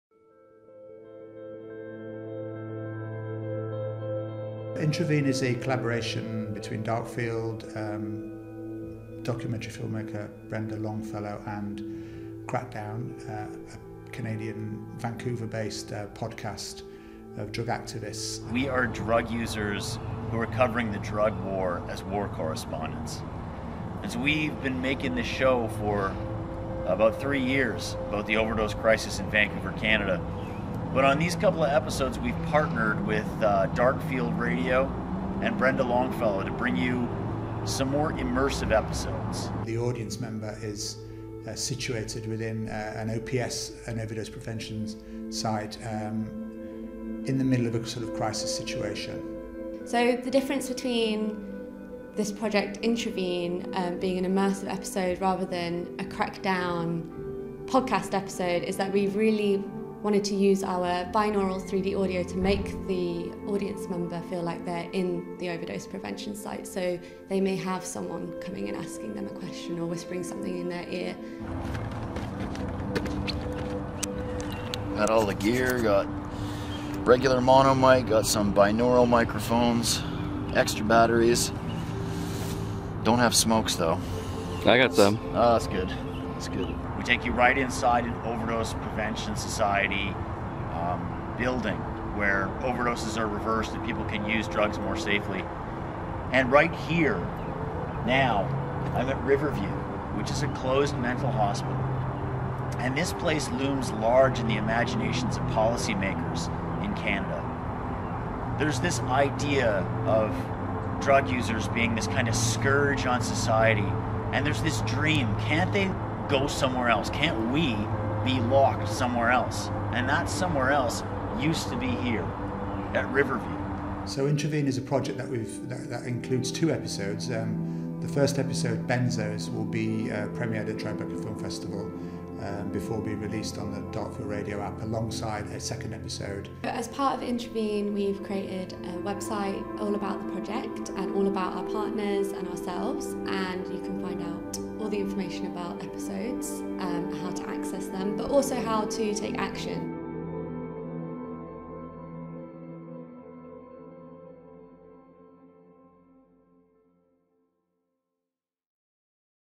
INTRAVENE Behind The Scenes - DARKFIELD, Crackdown & Brenda Longfellow
INTRAVENE is an immersive audio experience using binaural 360 degree sound to plunge listeners into the heart of the overdose crisis in Vancouver.
It is a groundbreaking partnership of immersive and documentary experts and drug user activists:  DARKFIELD, Crackdown and Brenda Longfellow.

Over 20 minutes, Episode One: BENZODOPE immerses listeners in the intense audio environment of the OPS where a user has inadvertently injected a hit contaminated with benzos. Episode One is in DARKFIELD's signature immersive style.

Over 45 minutes, Episode Two: RIVERVIEW confronts listeners with the ongoing spectre of institutionalising drug users. Episode Two is in Crackdowns' signature podcast style, with binaural sections.

INTRAVENE is an audience-ready project developed as part of the UK-Canada Immersive Exchange.